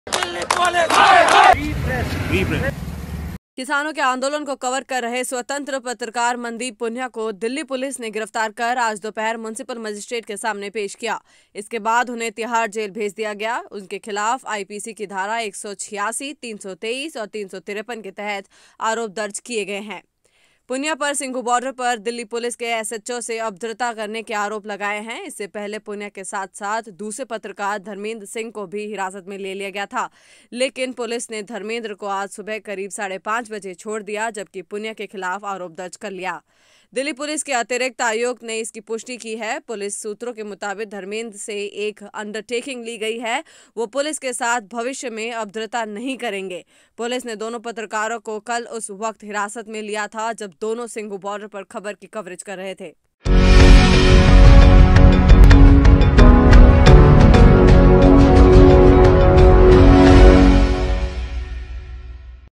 दिल्ली पुलिस ने पत्रकार को जेल में डाला, देखें क्या लगाए आरोप
किसानों के आंदोलन को कवर कर रहे स्वतंत्र पत्रकार मनदीप पुनिया को दिल्ली पुलिस ने गिरफ्तार कर आज दोपहर म्यूनिसिपल मजिस्ट्रेट के सामने पेश किया। इसके बाद उन्हें तिहाड़ जेल भेज दिया गया। उनके खिलाफ आईपीसी की धारा 186, 323 और 353 के तहत आरोप दर्ज किए गए हैं। पुनिया पर सिंघु बॉर्डर पर दिल्ली पुलिस के एसचओ से अभद्रता करने के आरोप लगाए गए हैं।